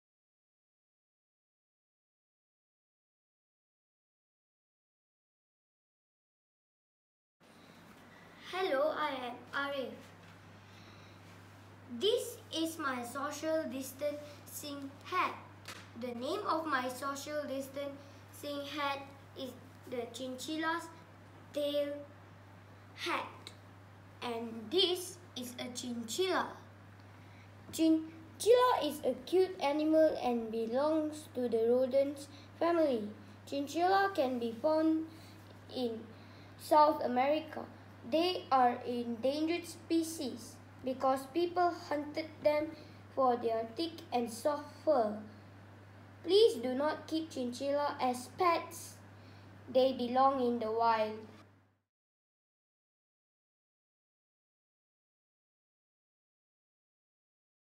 Social Distancing Hat Challenge (SD-HAT CHALLENGE)
Keeping children safe is our motto. Our Sekolah Rendah Rafflesia Social Distancing Hat Challenge (SD-HAT Challenge) has inspired a deep understanding of health awareness and how to maintain social distancing in a fun and creative way. As the new norm takes a toll on all of us, the young ones have boldly taken up the challenge to produce their own SD-HATs, ready to be worn once school re-opens.

Here, Arief Rafael from Std 2 is sharing his experience and inspiration in making his own SD-HAT!

Stay Strong, Stay Safe and Stay Home.